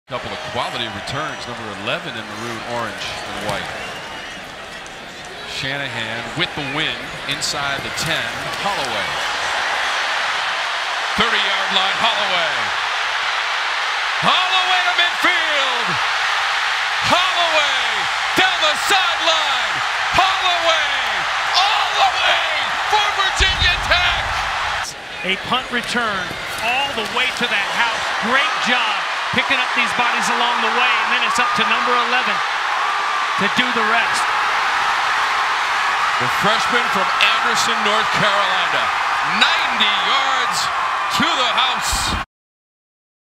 Virginia Tech's Tucker Holloway's Electric Punt Return | ACC Must See Moment
Check out Virginia Tech's Tucker Holloway send Lane Stadium into a frenzy with a 90-yard punt return for a score that is surely an ACC Must See Moment!